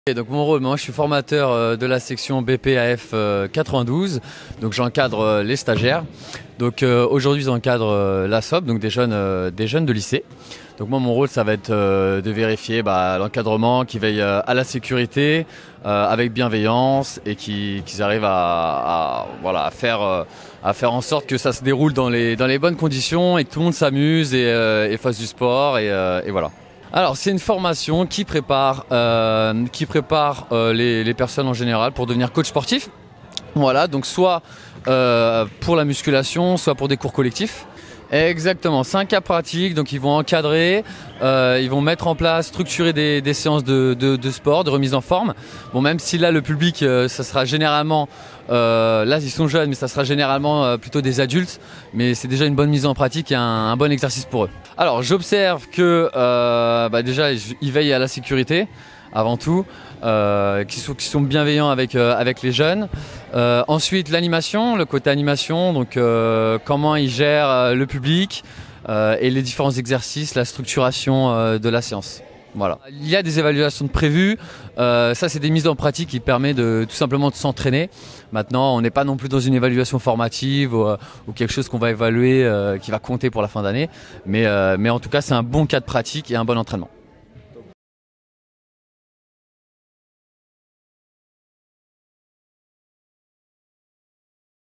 SOP 2022 : Le fitness, atelier de formation pour les BPJEPS
Au cours de cette Semaine Olympique et Paralympique au CREPS Île-de-France, les stagiaires en BPJEPS AF ont monté un module fitness, observé par les formateurs.